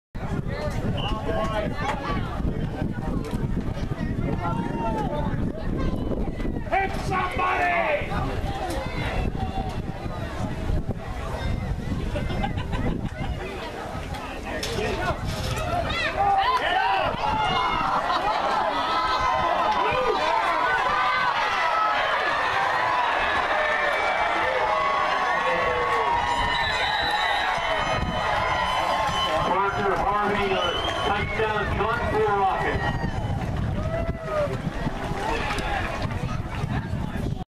Titusville Rockets touchdown run, part 2
This time #7 Parker Harvey gets the handoff, breaks a tackle, spins away from an Oil City player and burns the rest of the way for a 47 yard touchdown